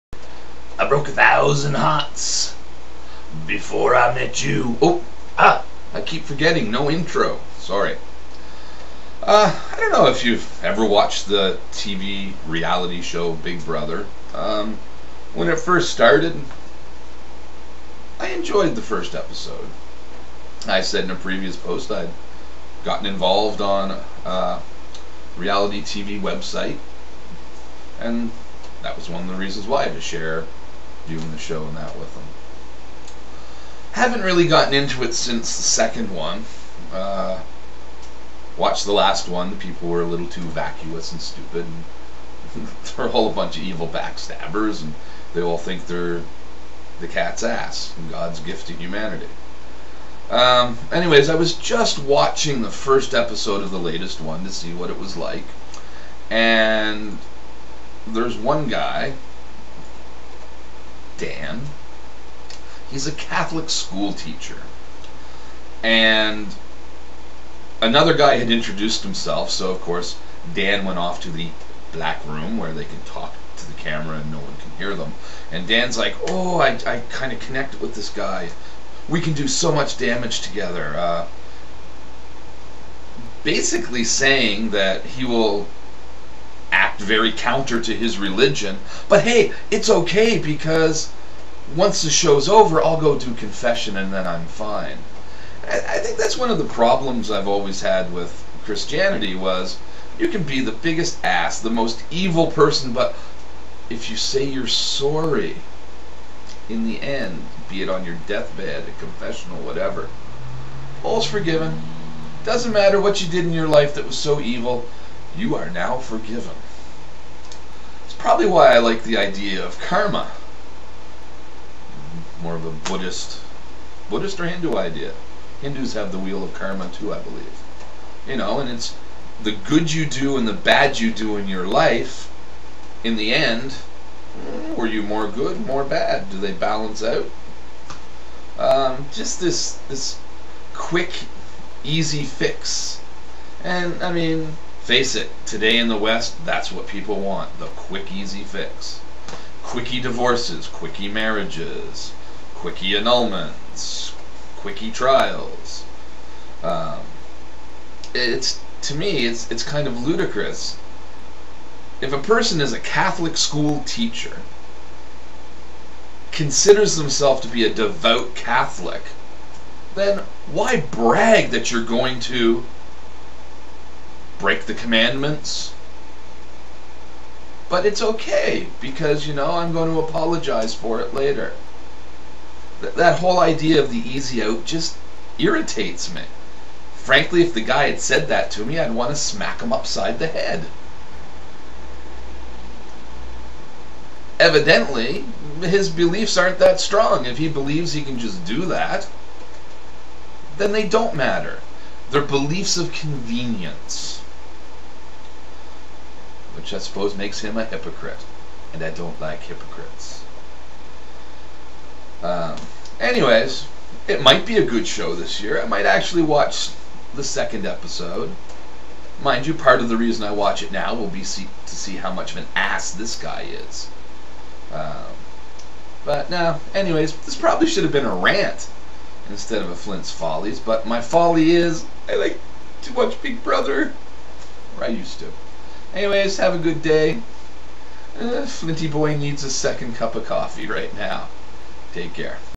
Flint's Follies 03 - Reality TV
Ok, I admit it...I liked(d) reality TV. :) But some Big Brother BS got my dander up.